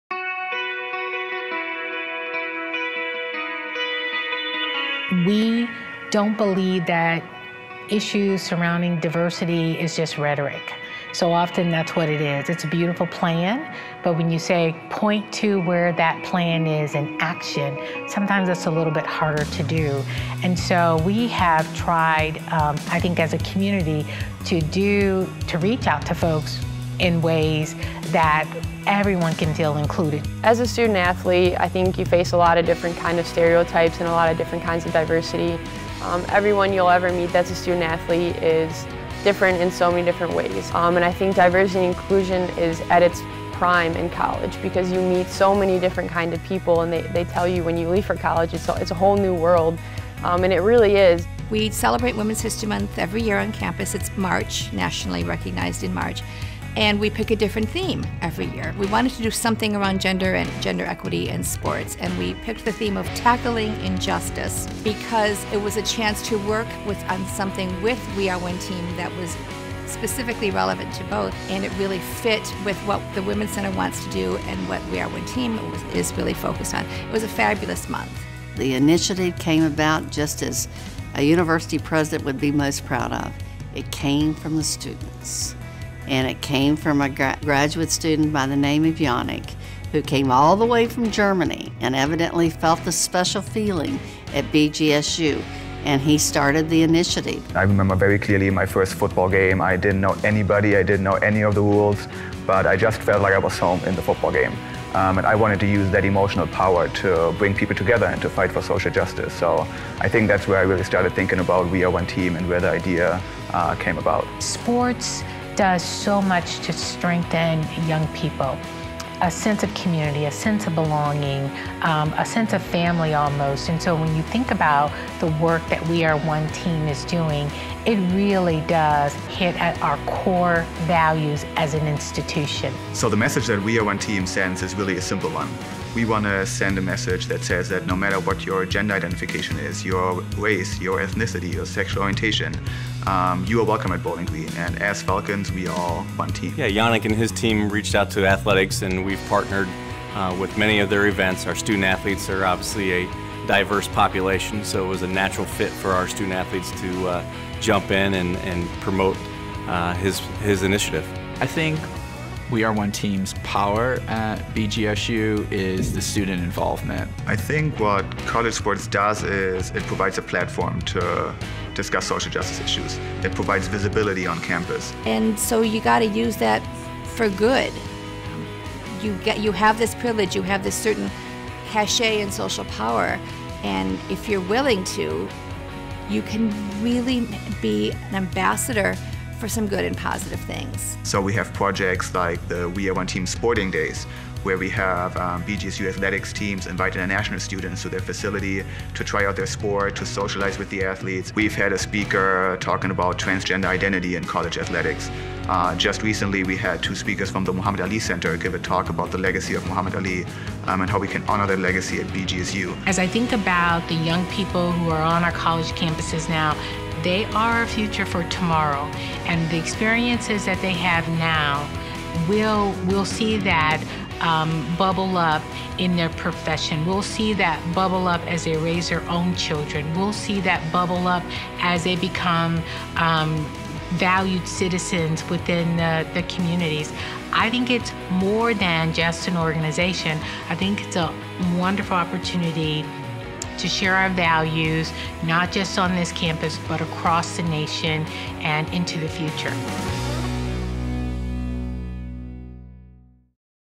2017 NCAA Diversity and Inclusion Award recipient Bowling Green State University
Bowling Green State University was named as the recipient of the 2017 NCAA and Minority Opportunities Athletic Association’s (MOAA) Diversity and Inclusion Award. BGSU was awarded for its efforts to promote diversity and foster a culture and environment of inclusion on campus.